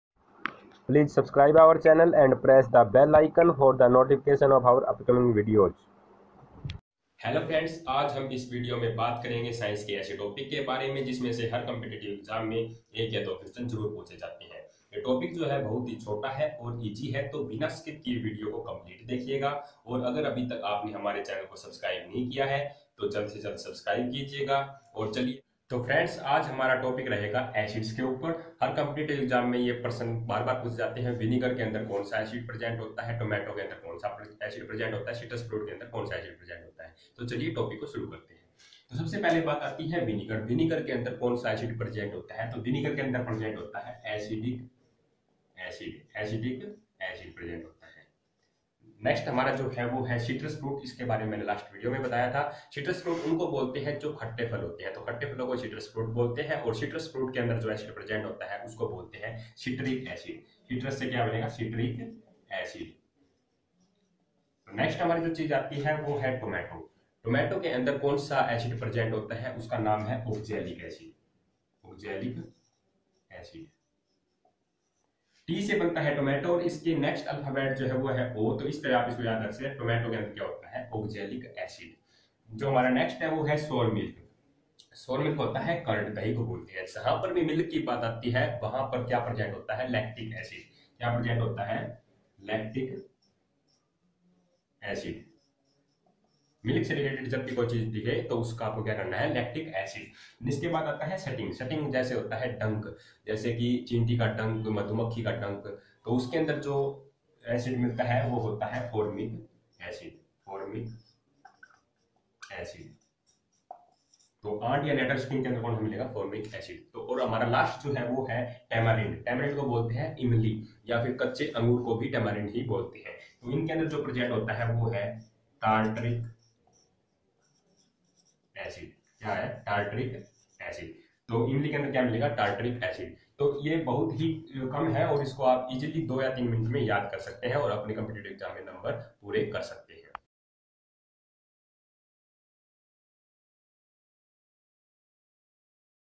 CTET important टॉपिक || Acids on your tips Science || @mii jogi
In this video we have discuss about the science topic which is very important for compitative exam like SSC CGL .
In this video , the acids present in fruits and vegetables which are mainly ask in every exam . Very short topic but also very important. Ctet special topic
Plz watch the video completly without skip.